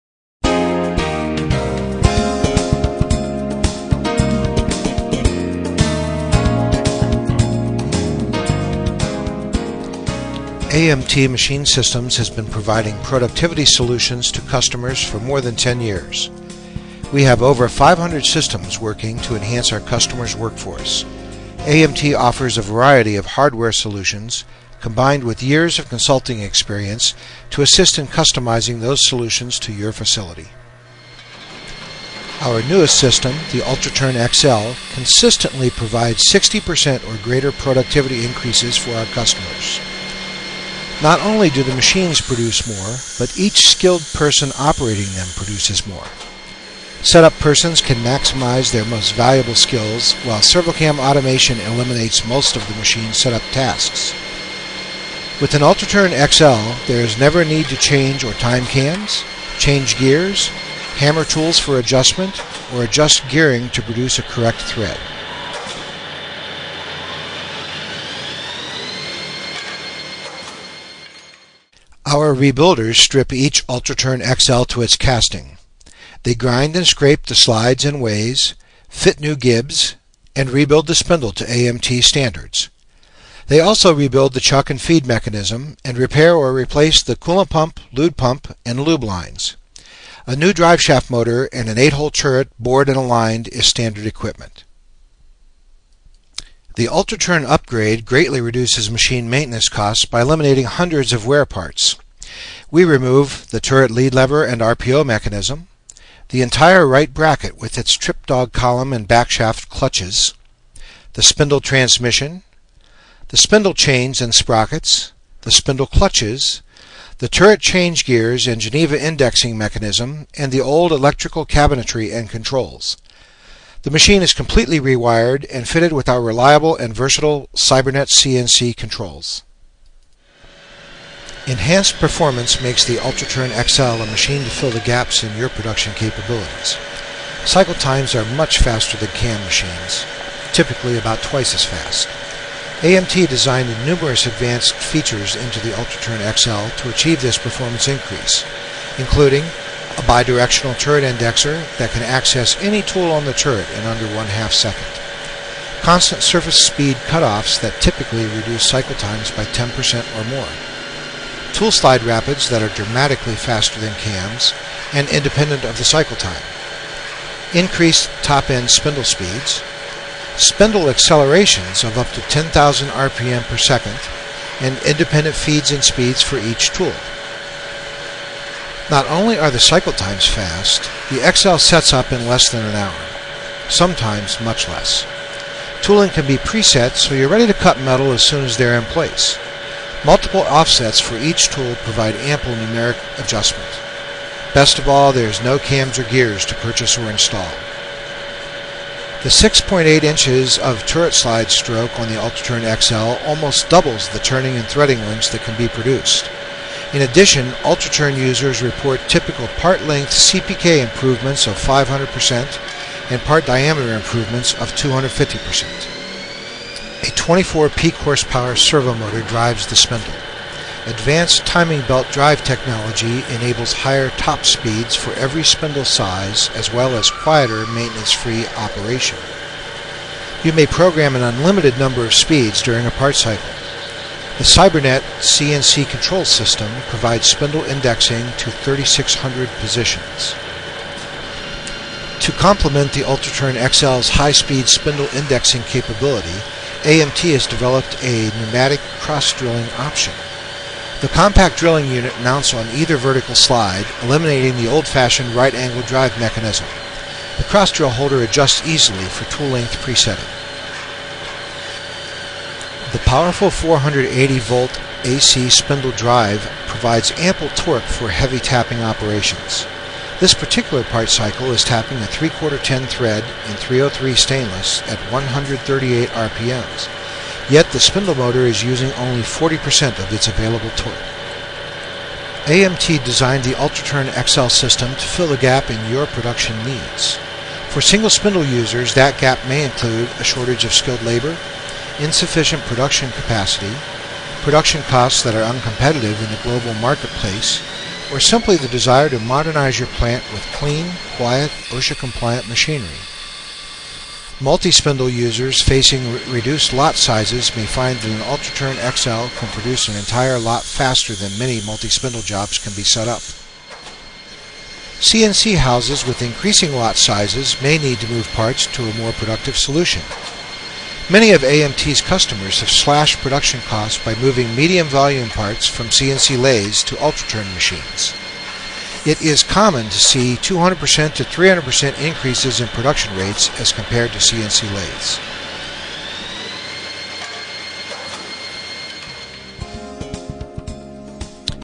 ServoCam UltraTurn XL operation
The UltraTurn XL is a high-speed cam-based CNC single spindle screw machine